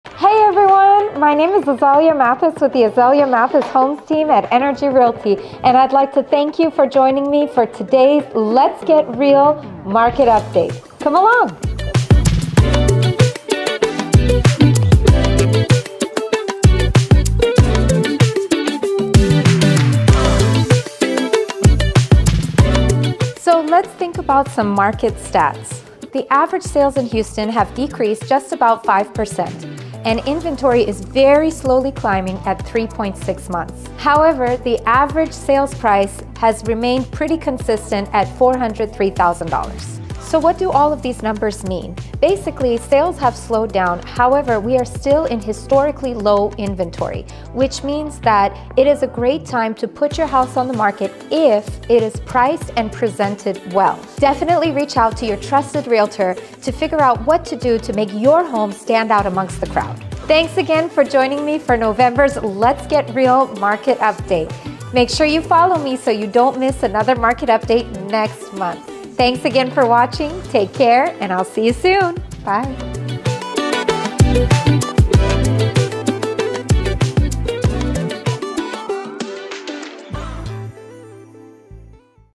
Let's Get Reel Market Update December 2023
Welcome to your December  2023 Let's Get Reel Market Update for Houston! Moving to Houston, Texas? Reach out to our team for a complimentary consultation!

Azalia Mathis, Team Lead of the Azalia Mathis Homes Team
Energy Realty